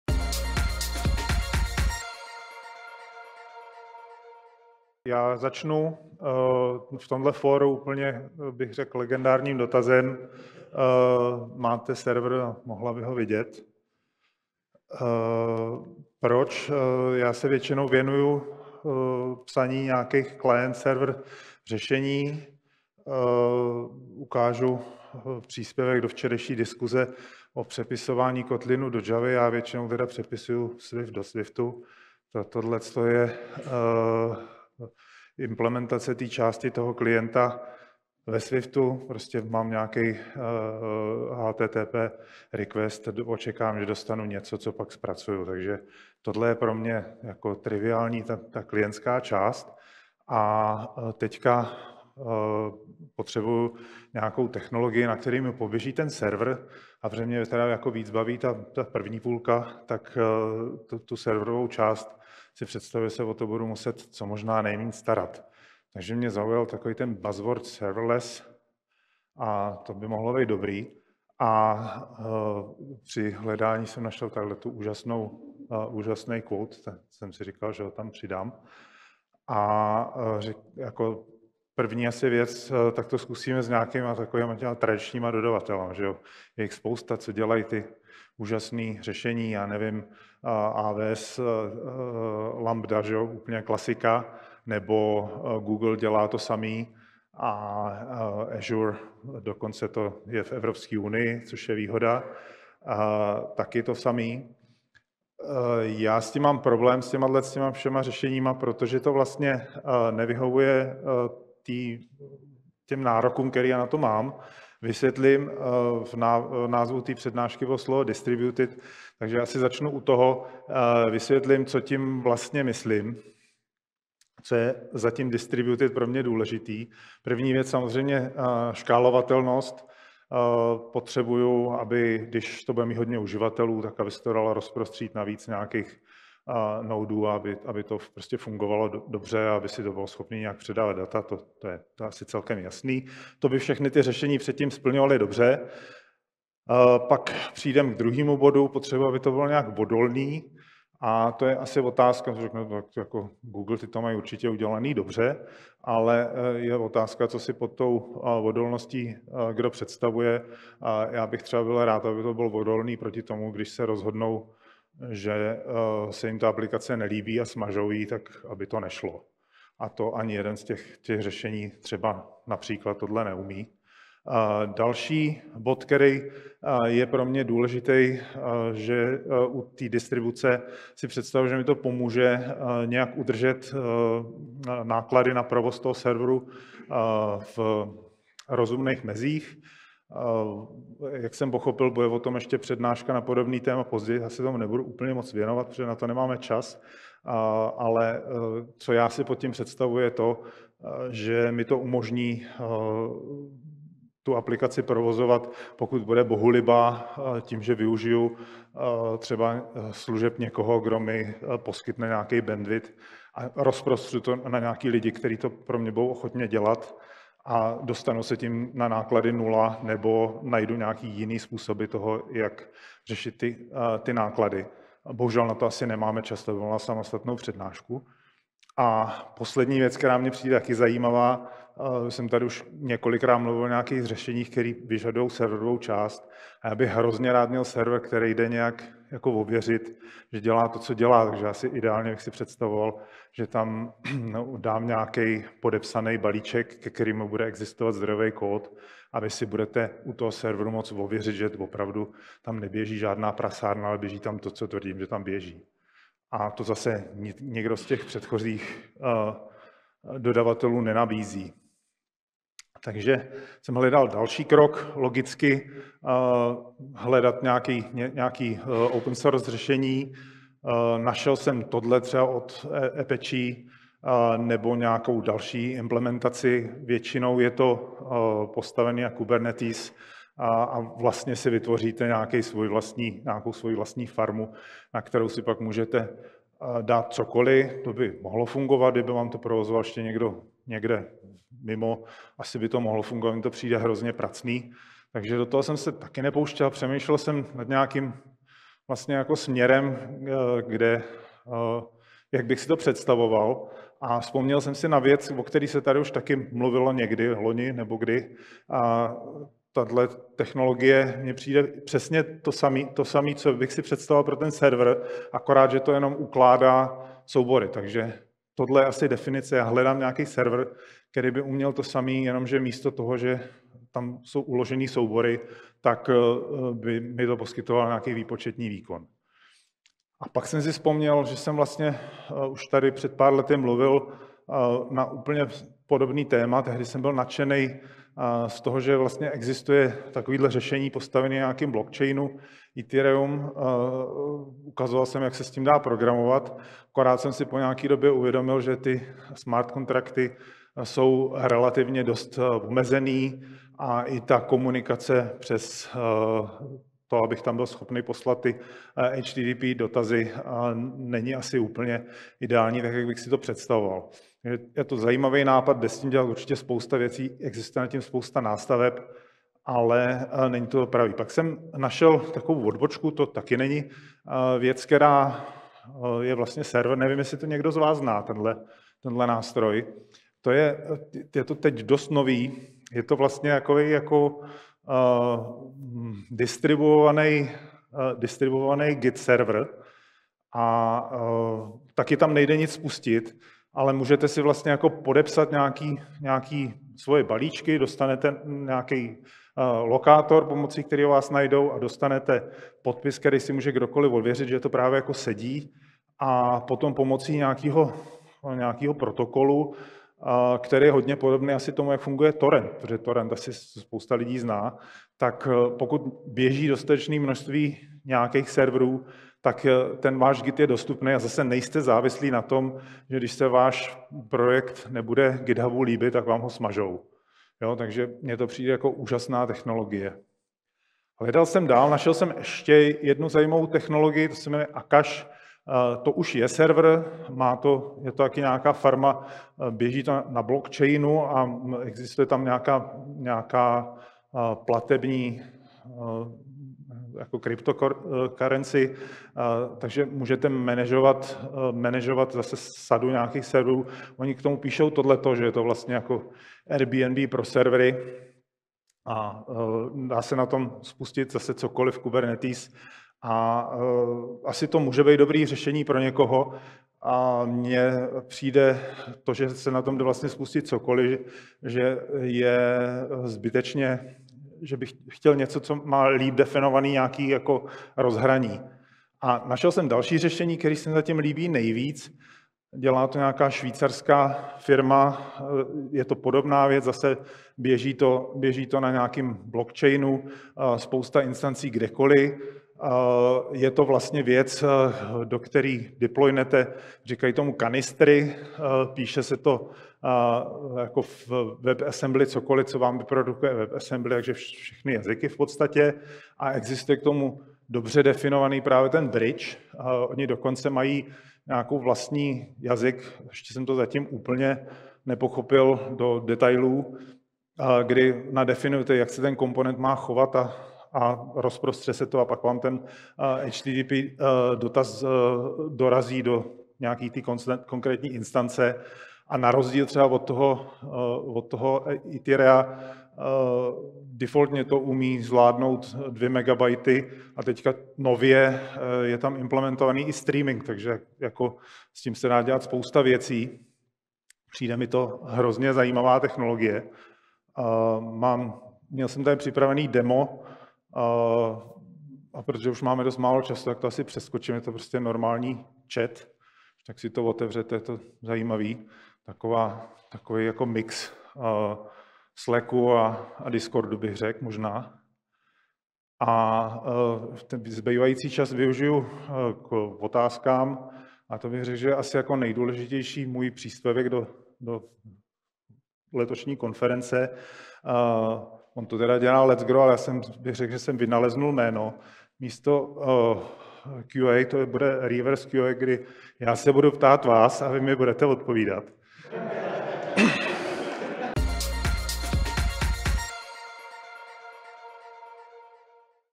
jOpenSpace 2024 - Pavel Lahoda - Distributed Serverless
Přednášející na začátku vysvětluje, že se často zabývá vývojem klient-server řešení a představuje část klientské implementace napsanou ve Swiftu. Poté se zamýšlí nad tím, jakou technologii použít pro serverovou část, přičemž preferuje řešení, o které se bude muset co nejméně starat. Zaujala ho myšlenka serverless, kterou ilustruje zmínkou o běžných poskytovatelích, jako jsou AWS Lambda, Google Cloud a Azure, ale narazil na problémy s jejich vhodností pro jeho požadavky.

Jedním z jeho klíčových požadavků je škálovatelnost a odolnost řešení, zejména pokud jde o nezávislost na poskytovateli, který by mohl kdykoliv aplikaci odstranit. Také zdůrazňuje důležitost snížení provozních nákladů a hledání způsobů, jak aplikaci provozovat s minimálními náklady. Dále se věnuje myšlence, že by bylo ideální, kdyby serverové řešení bylo možné ověřit z hlediska toho, co běží na serveru, a tak zajistit, že nedojde k neoprávněným zásahům.

Přednášející poté popisuje různé technologie, které zkoumal, včetně open-source řešení založených na Kubernetes a blockchainu. Zmiňuje například technologie Akash a Ethereum, ale naráží na jejich omezení, jako je například komplikovaná komunikace přes smart kontrakty.

Nakonec představuje technologii od švýcarské firmy, která pracuje s WebAssembly a blockchainem. Toto řešení nabízí možnost nasazování kanistrů, které fungují jako malé distribuované výpočetní jednotky, a podporuje zpracování HTTP dotazů. Zdůrazňuje, že tato technologie je pro něj zatím nejzajímavější, protože umožňuje škálování a nově implementuje i streaming. Na závěr přednášky se věnuje otázkám a odpovědím s publikem a představuje koncept „revers Q&A“, kdy se ptá posluchačů a oni mu odpovídají.